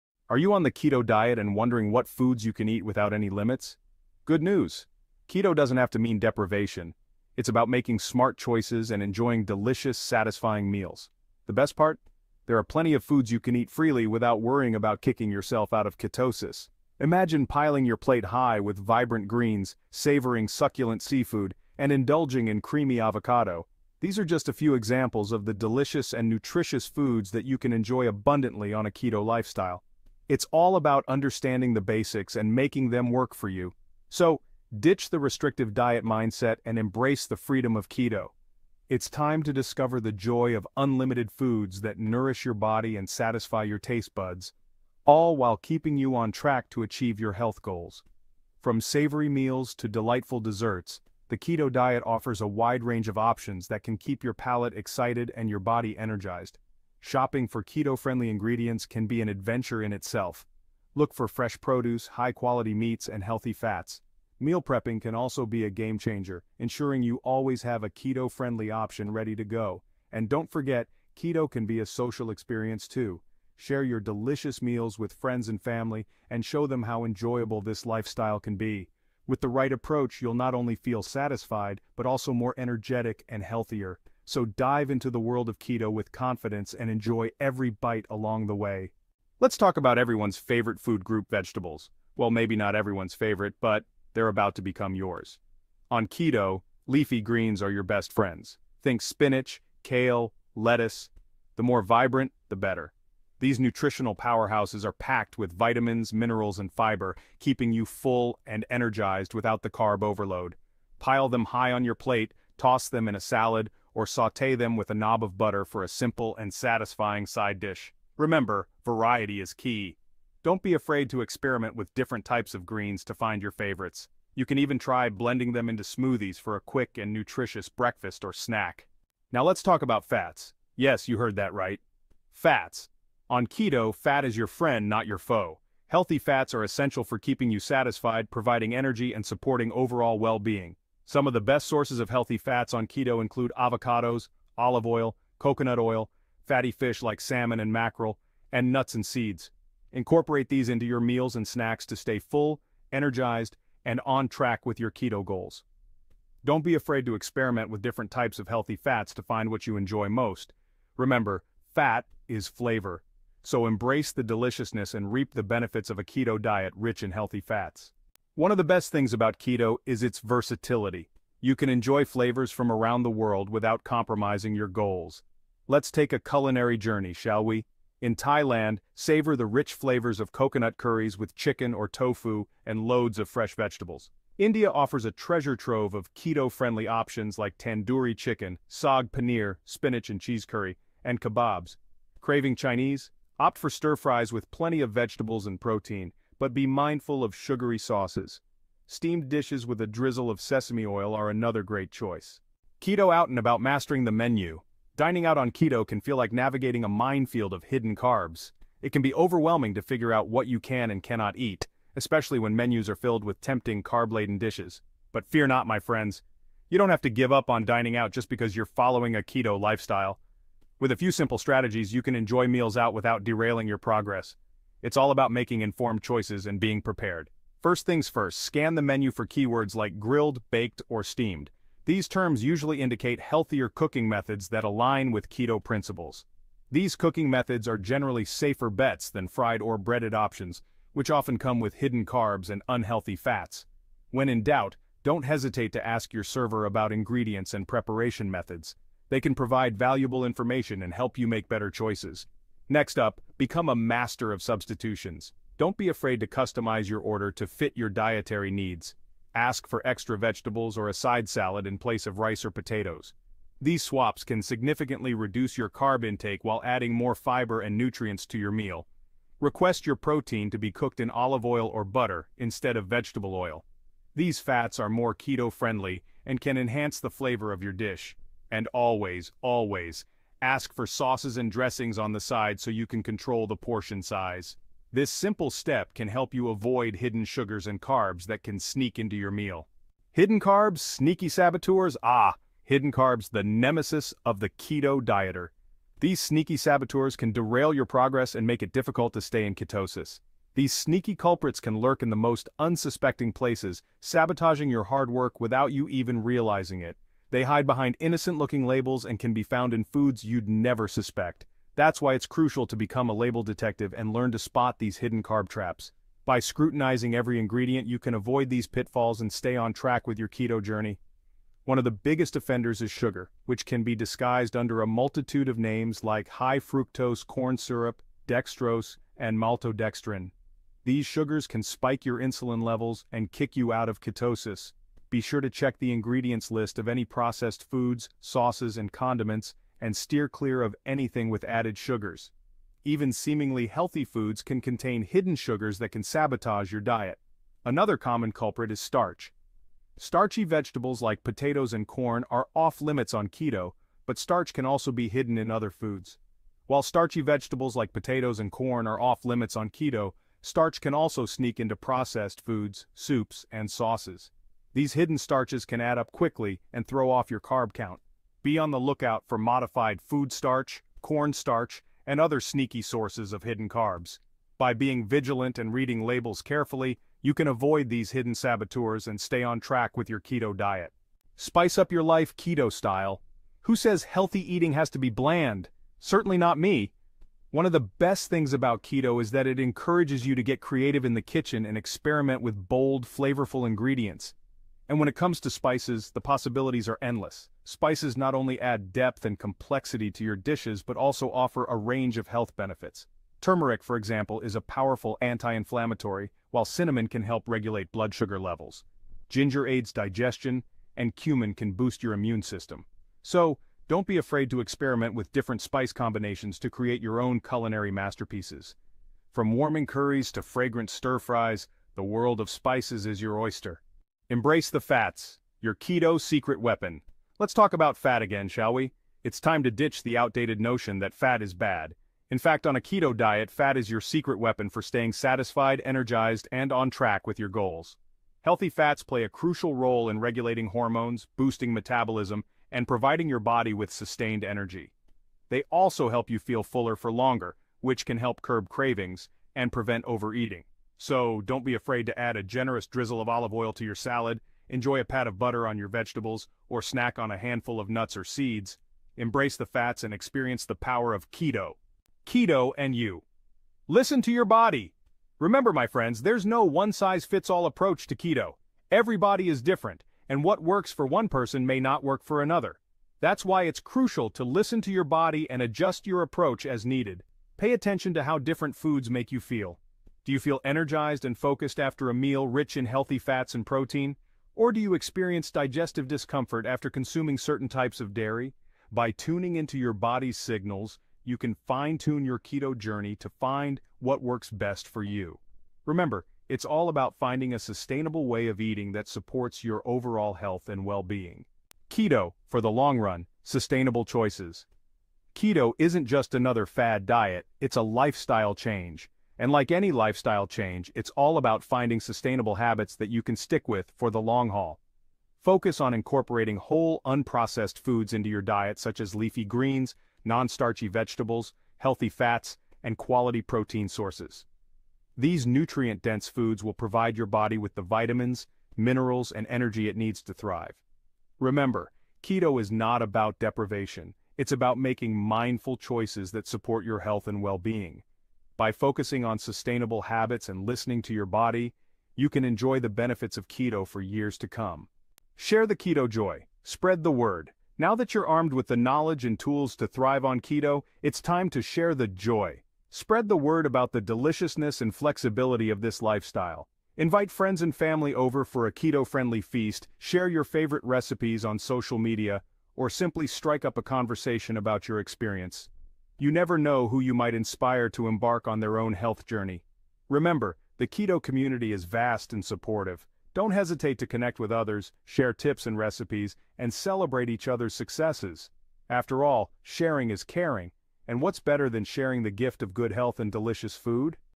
What foods can I eat unlimited on keto ?
Discover the ultimate guide to enjoying unlimited foods on Keto without breaking your diet! In this video, we dive deep into what foods you can eat unlimited on keto, providing a list of delicious options that you can enjoy freely. Ever wondered what fried foods you can eat on keto or how to navigate your favorite international cuisines like Thai food, Indian food, Chinese food, and Mexican food while staying within your Keto guidelines? We've got you covered! Learn what foods you can not eat on keto and get tips on how to stick to low carb foods that keep you in ketosis. Whether you're just starting out or looking to expand your keto foods to eat list, this video will help you make informed choices. Plus, find out what foods to eat to get shredded while enjoying every bite!

OUTLINE:
00:00:00 Eat Your Fill!
00:01:29 Green Light, Go for Green!
00:02:09 Dive into Delicious Fats!
00:02:48 A Culinary Adventure!
00:03:22 Mastering the Menu!
00:04:49 Sneaky Saboteurs!
00:06:21 Spice Up Your Life, Keto Style!
00:07:04 Your Keto Secret Weapon!
00:07:42 Listen to Your Body!
00:08:21 Sustainable Choices!
00:09:05 Spread the Word!